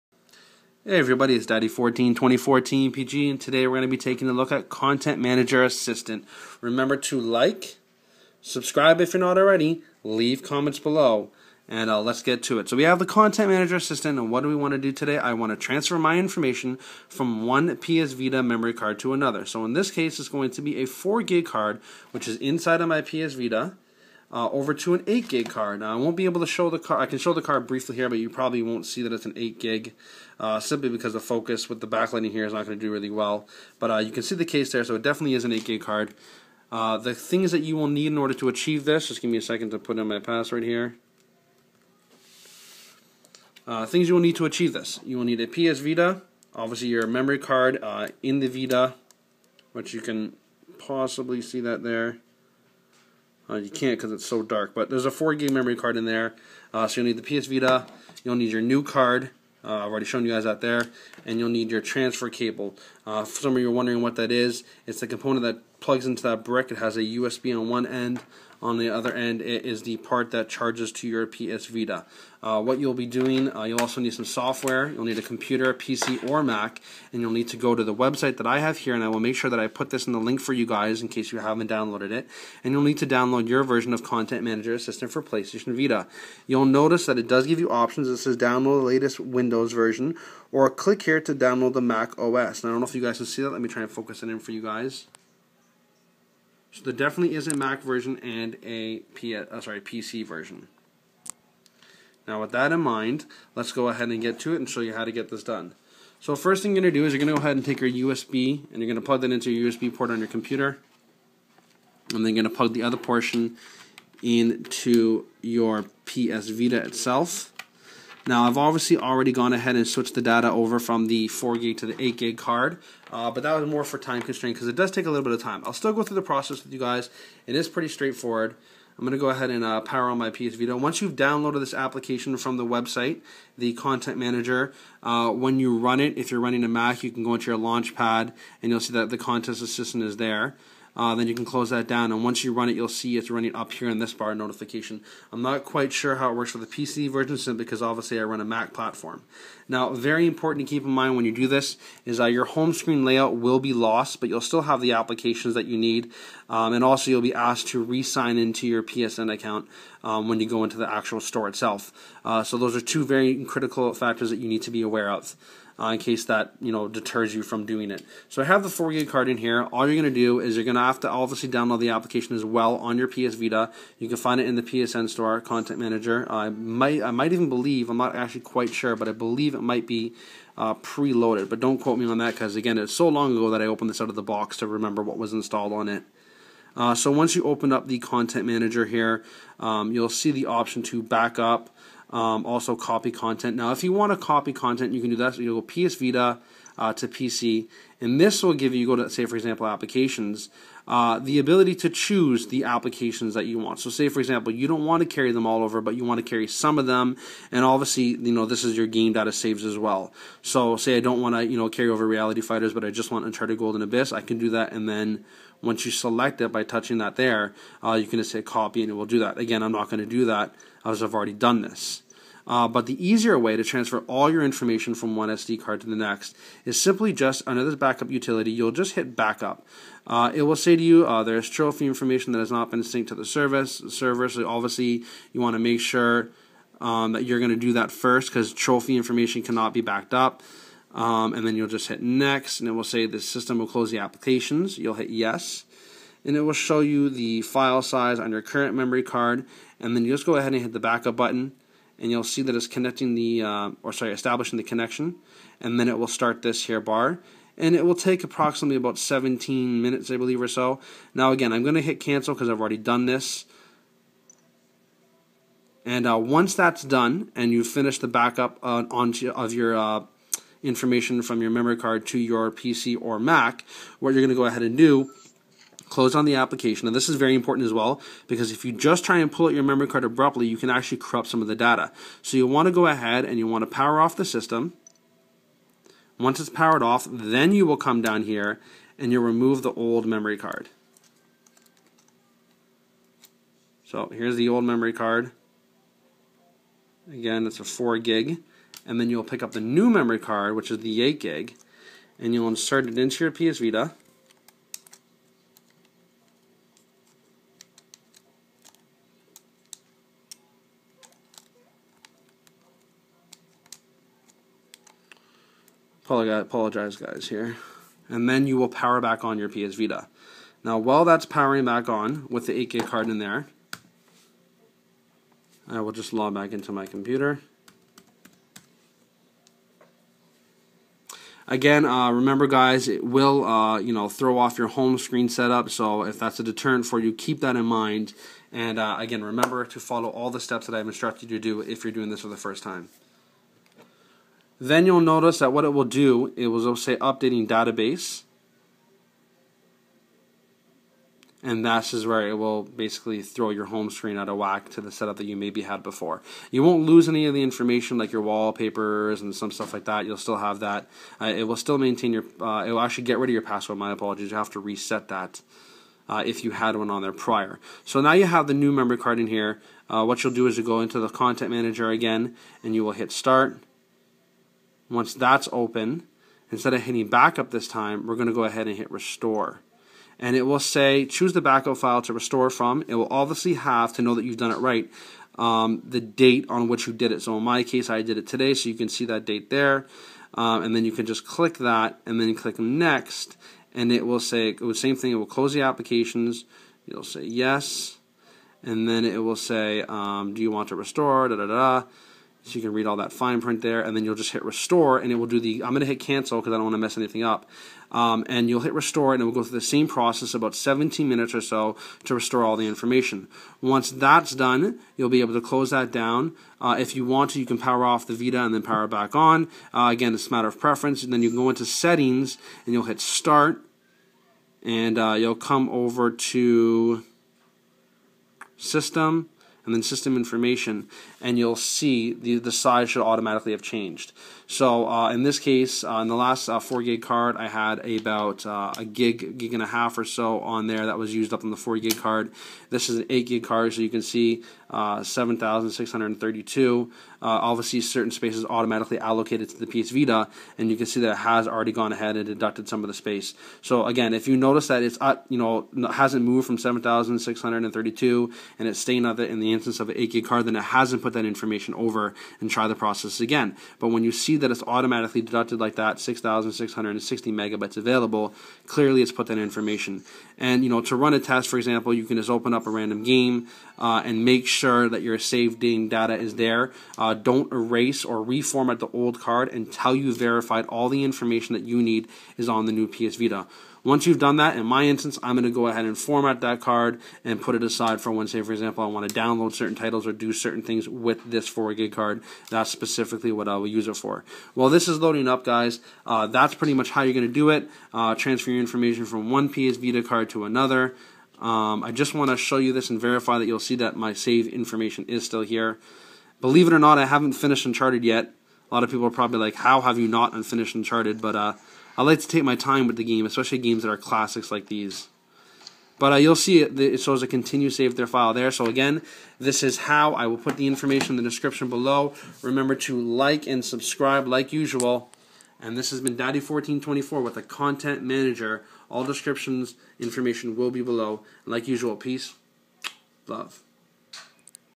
Content manager/ swapping memory cards
Here is a how to, I felt would be helpful for those looking to upsize there memory cards and don't want to lose their content. Watch and find out the answer to your questions.